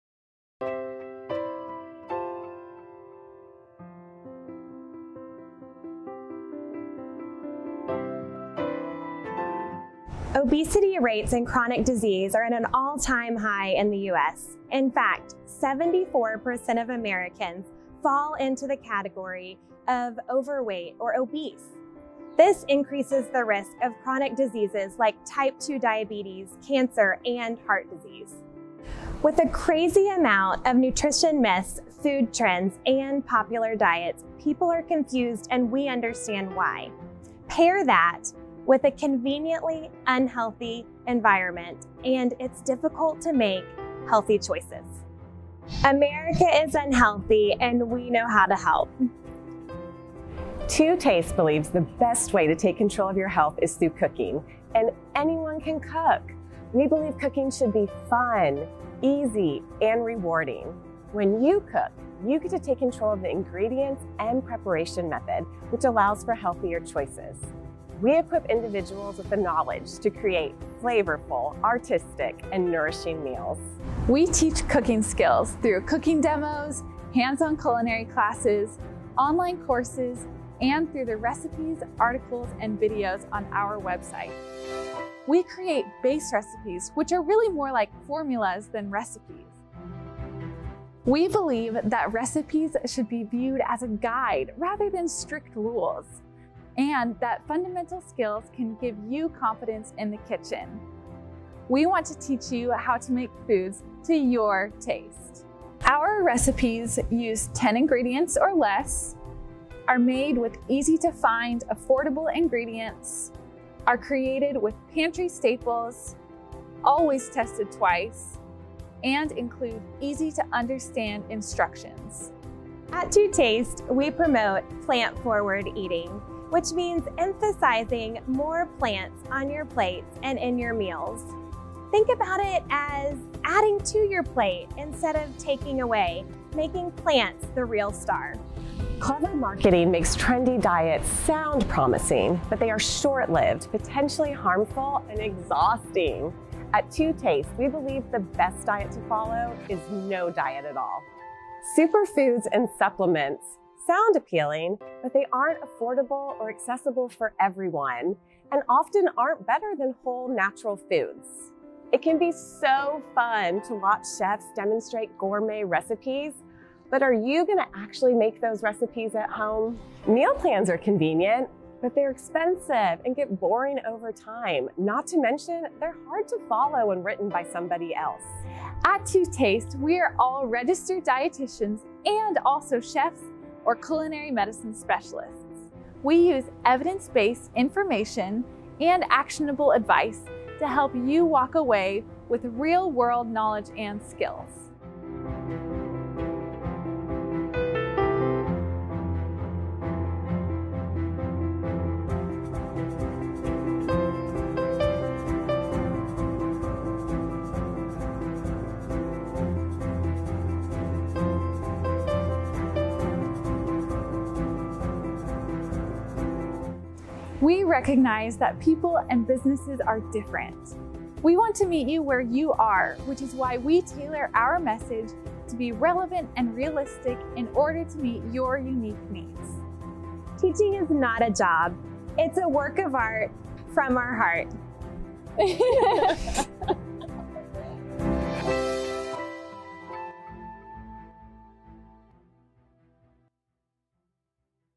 ABOUT TO TASTE
To Taste believes that anyone can take control of their health through cooking!

Our goal is to teach culinary techniques through dozens of base recipes that allow you to customize each recipe to your taste.

We equip individuals with the knowledge to create flavorful, artistic, and nourishing meals.